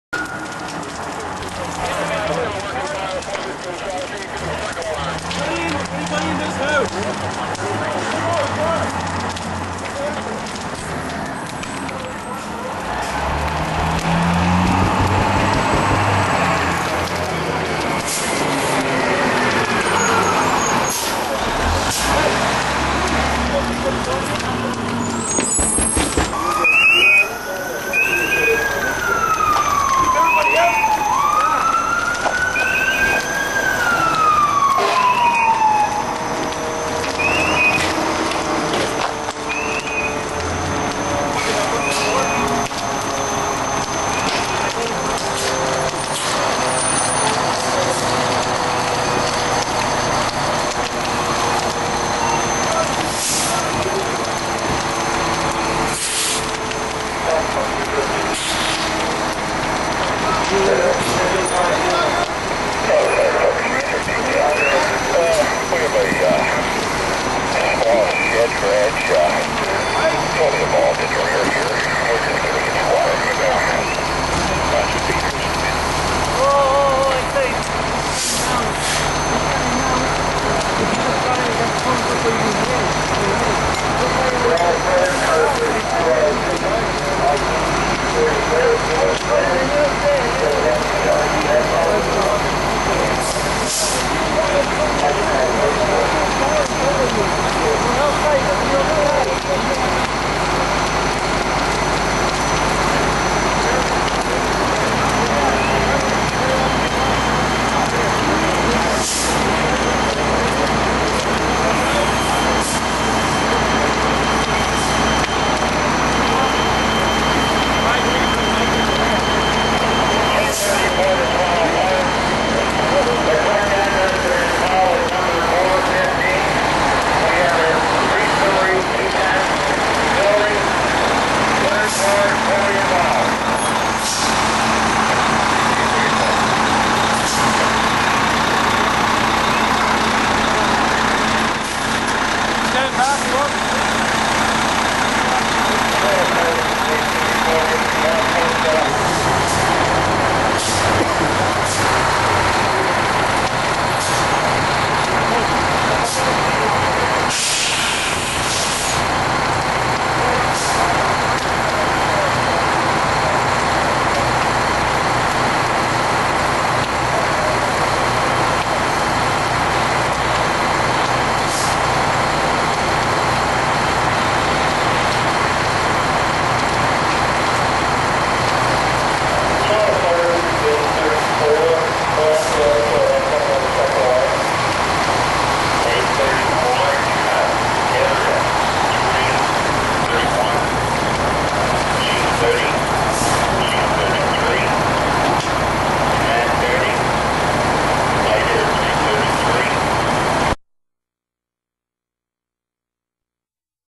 Toronto fire first in
Video of first arriving rigs goes to a 2nd alarm